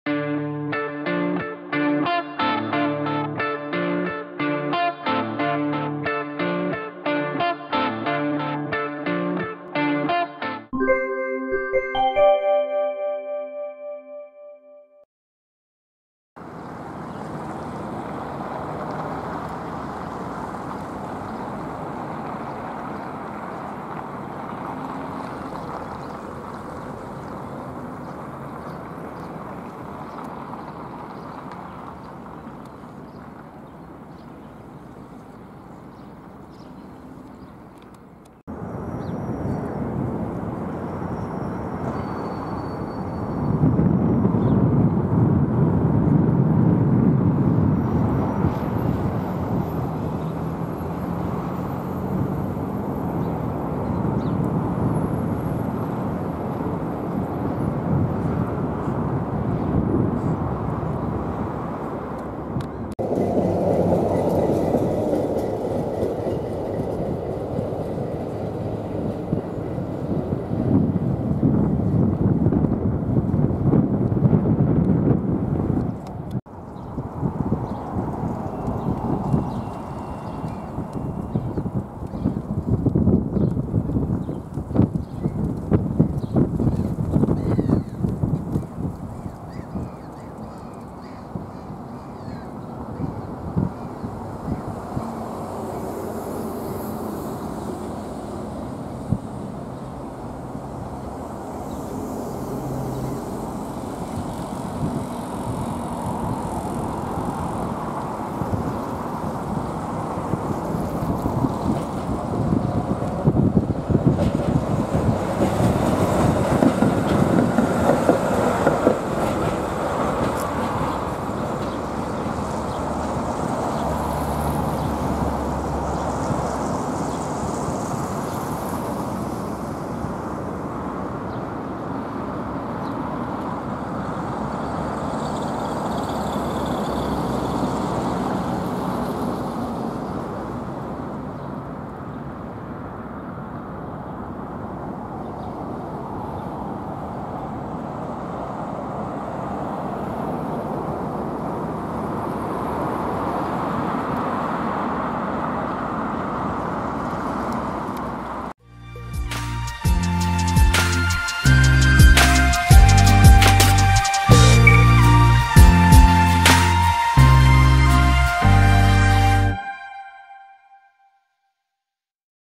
Учебный! Трамвай ПС70 №8335 по маршруту №У на проспекте Ветеранов, Санкт-Петербург апрель 2023 года
Трамвай ПС70 №8335 (Приписан в ГУП "ГорЭлектроТранс", трамвайный парк №8) маршрут №У - учебный.

Проспект Ветеранов.
Российская Федерация, Санкт-Петербург.
14.IV.2023
Закат снят 12.IV.2023